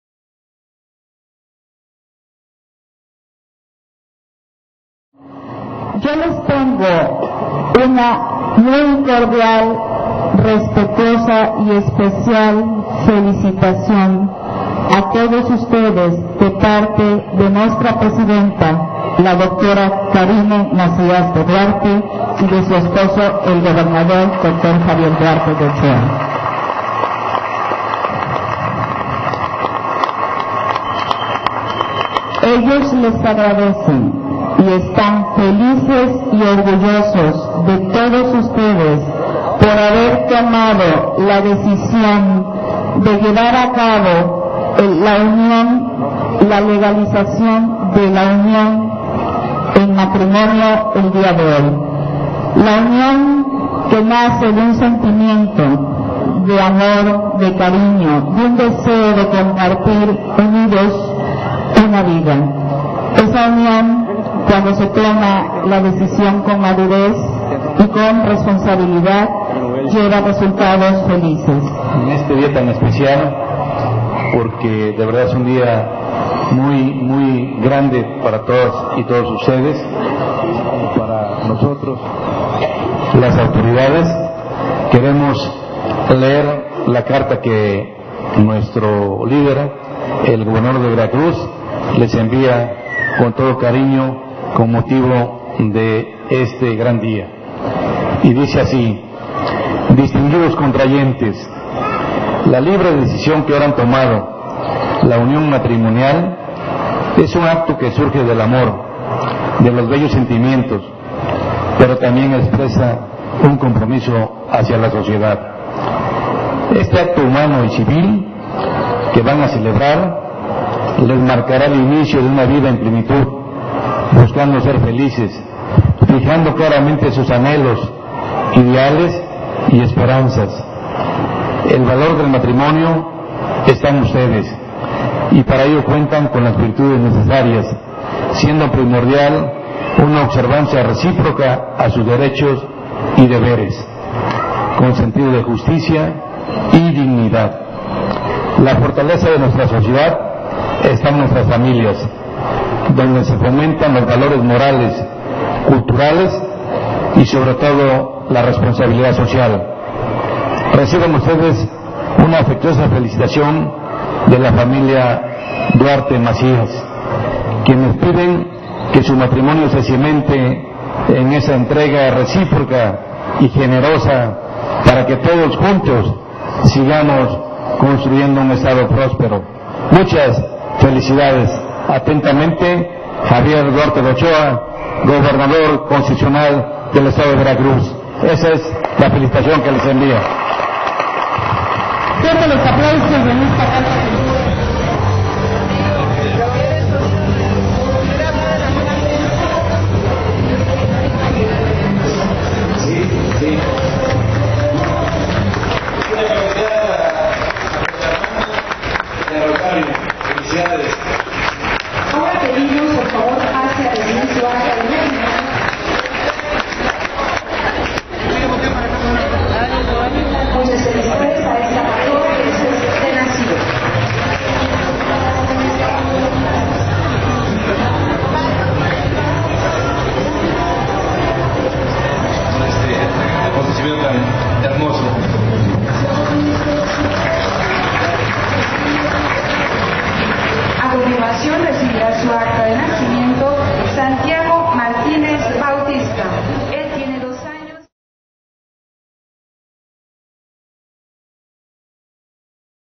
Matrimonios colectivos - Córdoba Veracruz
Celebran matrimonios colectivos y asentamientos extemporaneos en la ciudad de Córdoba Veracruz con motivo del 14 de febrero